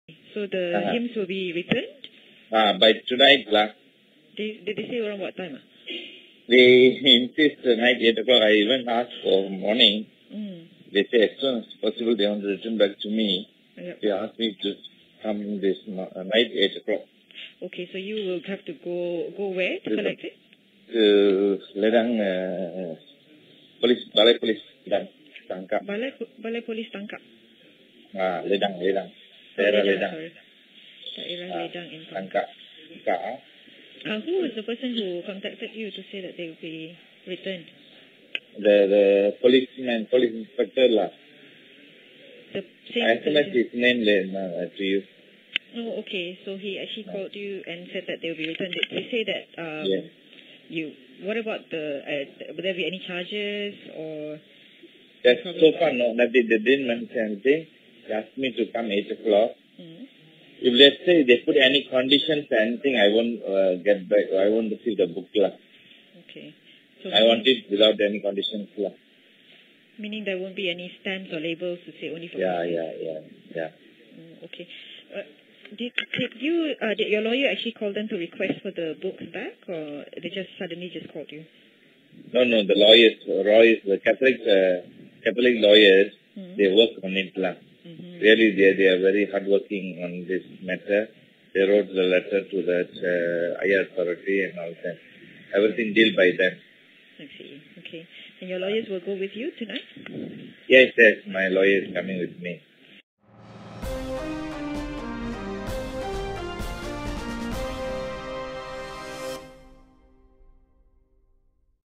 Cops to return hymnals, priest wants no catches
The police will return the 31 hymn books with the word 'Allah' seized in Johor a fortnight ago to the Catholic church tonight. However, Father Cyril Mannayagam said he will not accept them if they are stamped or a limit is set on their circulation.